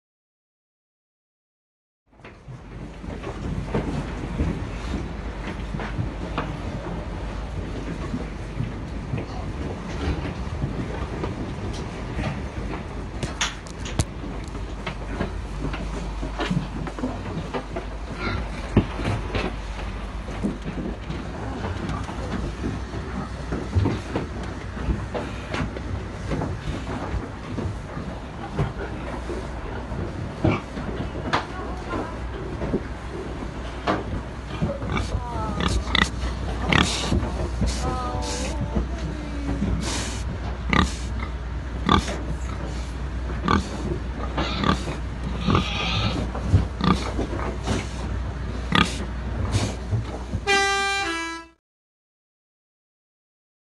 F A Gills Vigil 31/05/2018
West midlands Animal Save bear witness and document the final moments of Pigs headed for slaughter.